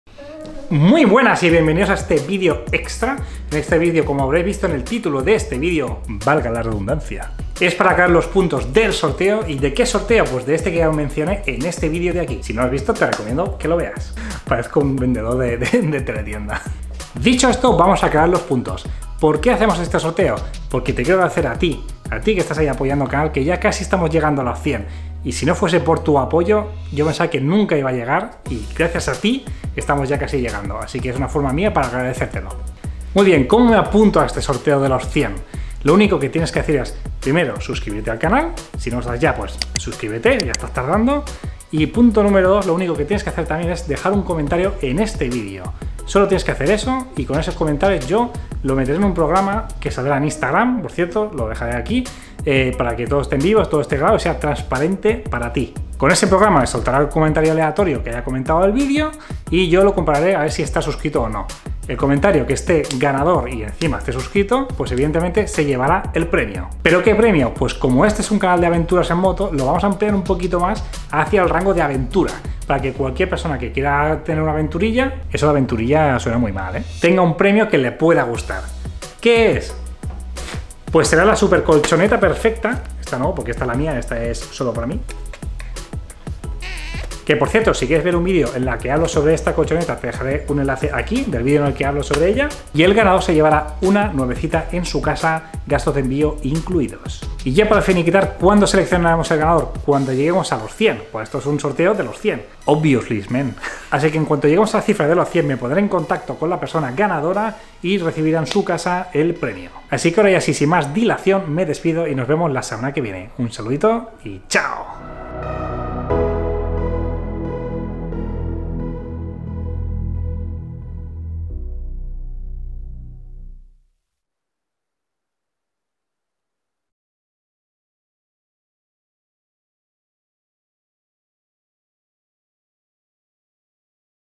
¡¡ 🍾ANUNCIO SORTEO 100 SUBS 🍾!!
Pasos para apuntarte al sorteo:
2. Dejar un comentario en este mismo vídeo.

Opcional:
Si además quieres seguirme en Instagram: IronRiderWH te lo agradeceré y podrás ver en vivo el sorteo!